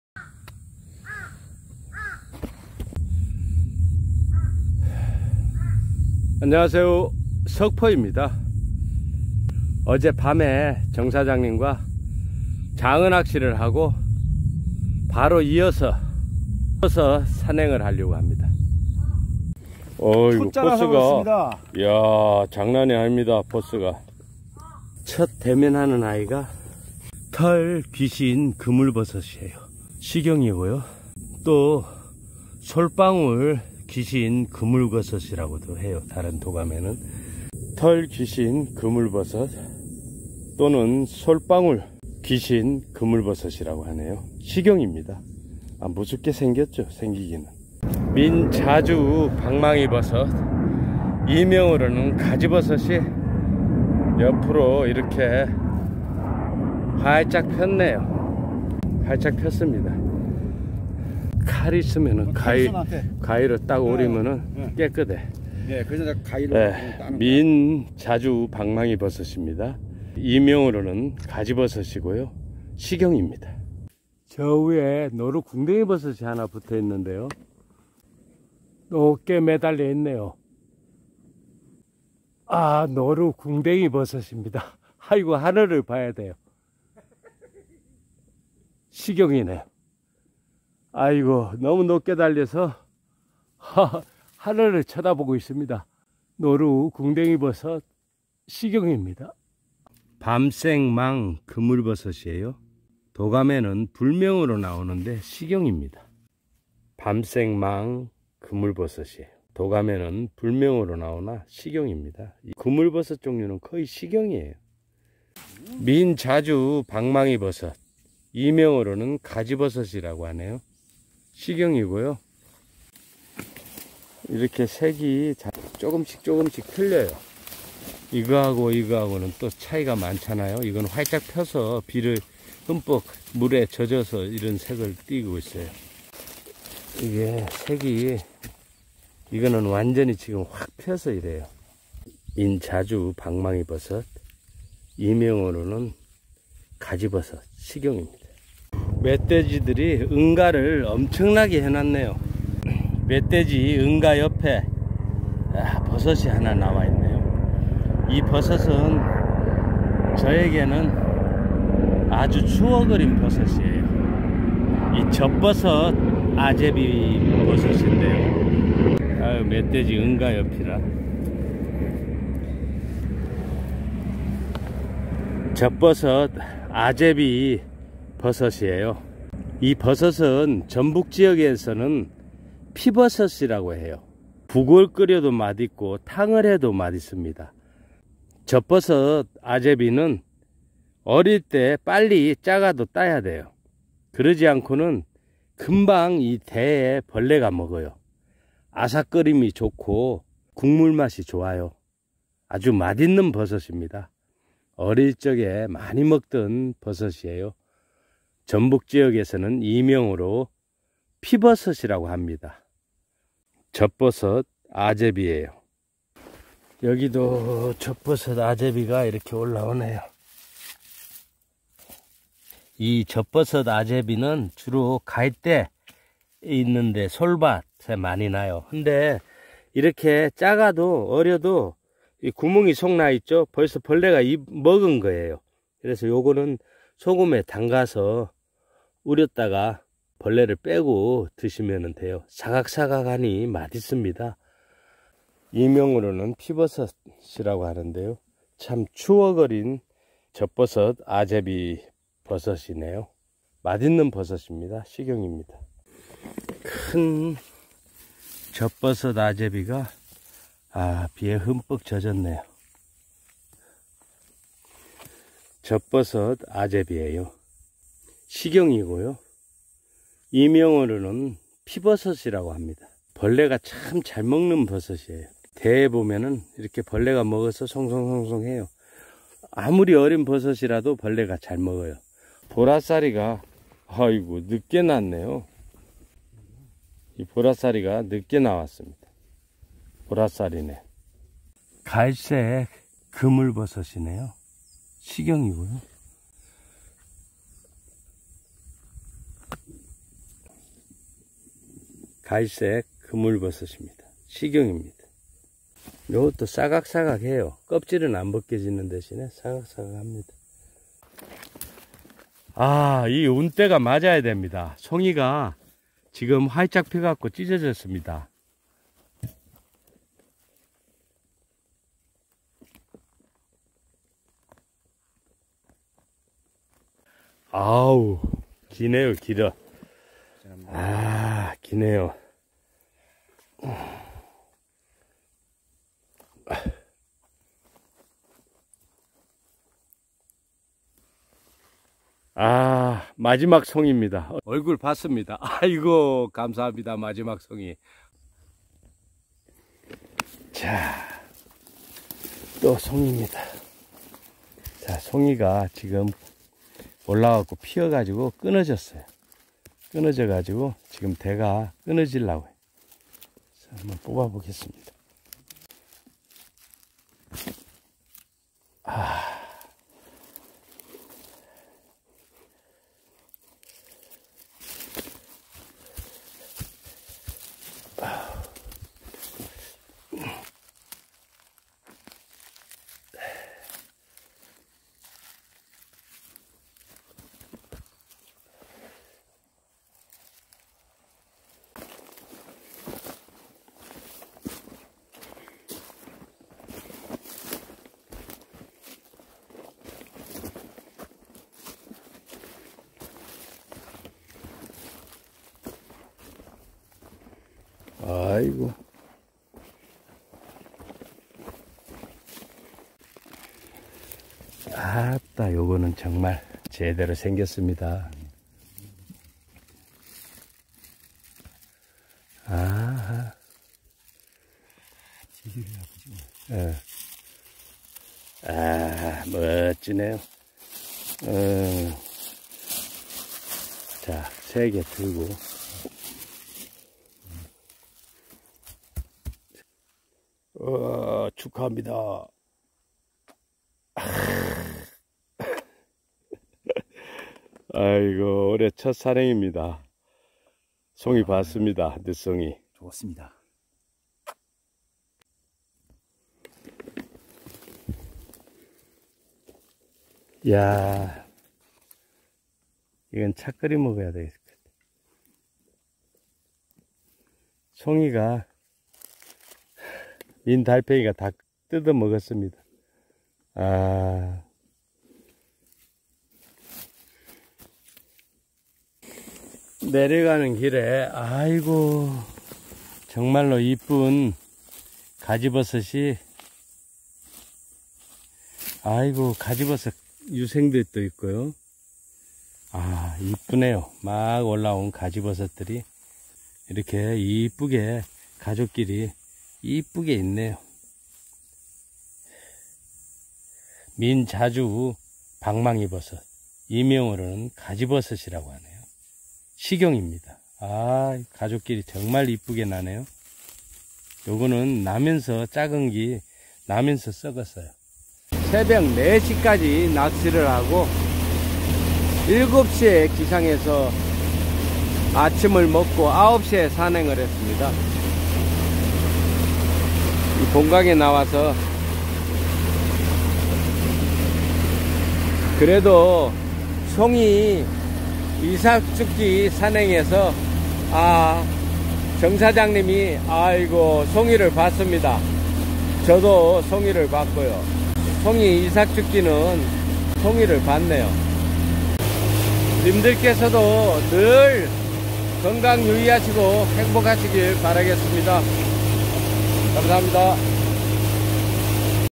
[약초]송이버섯 산행 (충북)
송이버섯 이삭줍기 산행에서 털귀신그물버섯(솔방울귀신그물버섯).민자주방망이버섯 (가지버섯).노루궁뎅이버섯. 밤색망그물버섯(밤색갓그물버섯).젖버섯아재비(피버섯).보라싸리버섯. 암갈색새그물버섯 (임시이름)을 만나보았네요.